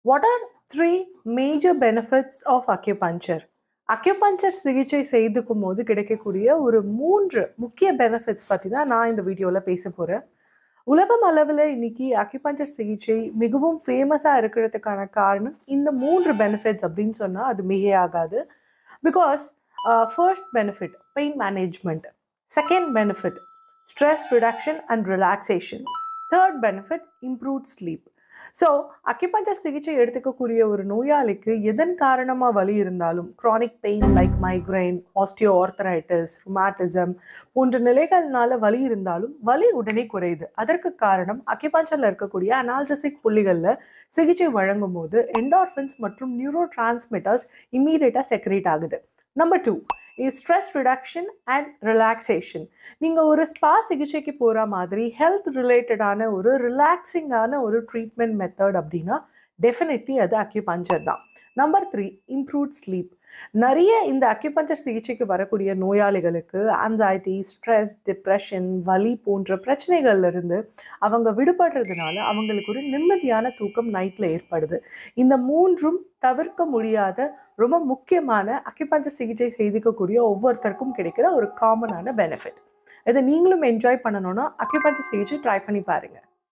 What are the Three Major Benefits of Acupuncture????
"Dive into the world of acupuncture with Dr. Nargis Wahaab in her Health Mantra series! 🌟 With over two decades of expertise, she demystifies the myths around acupuncture side effects. According to her, there are no side effects—only healthy side effects! ✨ Discover the healing journey with Dr. Nargis and embrace a holistic approach to well-being. AcupunctureWisdom HealthMantra HolisticHealing"